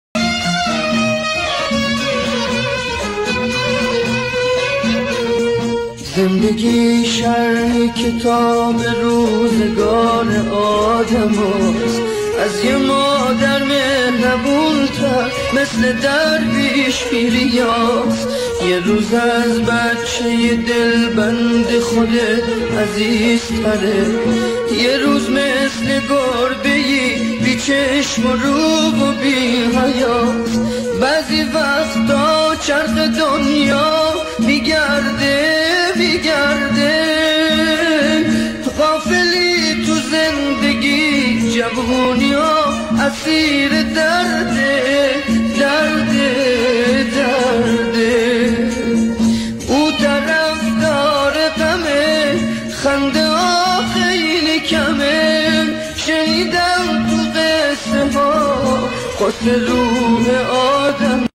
Taft
Taft Village, Yazd has many spectacular cities and villages; one of these sights is a cool city surrounded by Rocky Mountains. A city that can shock us with its nature in the middle of the desert. On the one hand, the slopes of the mountains are full of snow and on the other, desert villages. That’s why the trip to Taft should wait for any weather was.